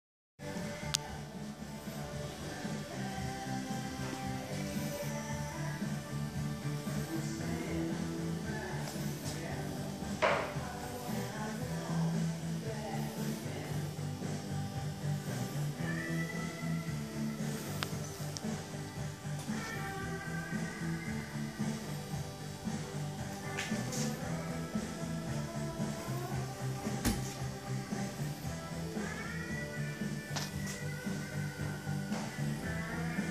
Hydrographic painted Honda Fireblade tank.
My first large hydrographics project.  Honda Fireblade tank in snakeskin and skulls.